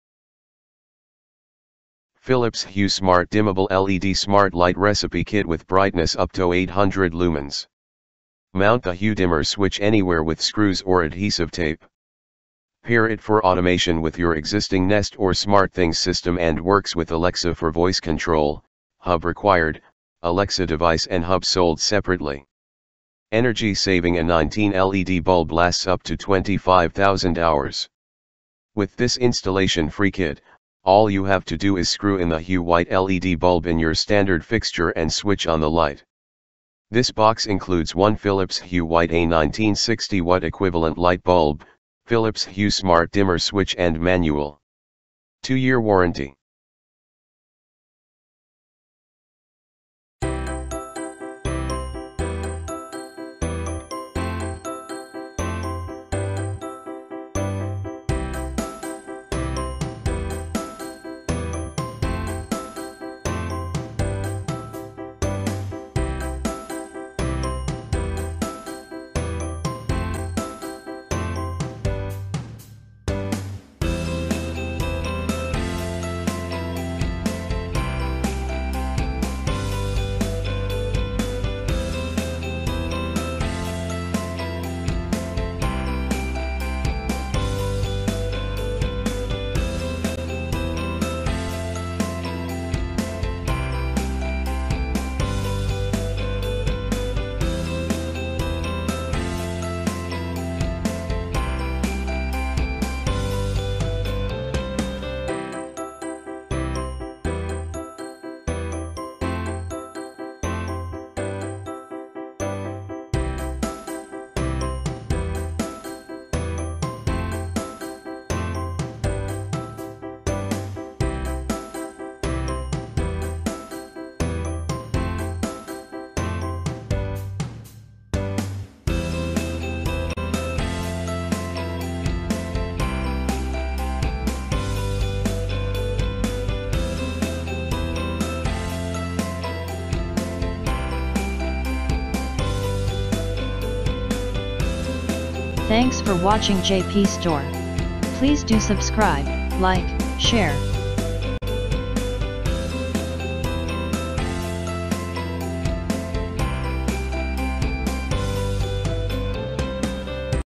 Philips Hue Smart Dimmable LED Smart Light Recipe Kit, Installation
Philips hue smart dimmable LED smart light recipe kit with brightness upto 800 lumens
Mount the Hue Dimmer Switch anywhere with screws or adhesive tape
Pair it for automation with your existing Nest or SmartThings system & works with Alexa for voice control (hub required, Alexa device and hub sold separately)
Energy saving a19 LED bulb lasts up to 25000-hours
With this installation free kit, all you have to do is screw in the Hue white LED bulb in your standard fixture and switch on the light
This box includes one Philips Hue White A19 60-Watt equivalent light bulb, Philips Hue Smart Dimmer Switch & manual
2 year warranty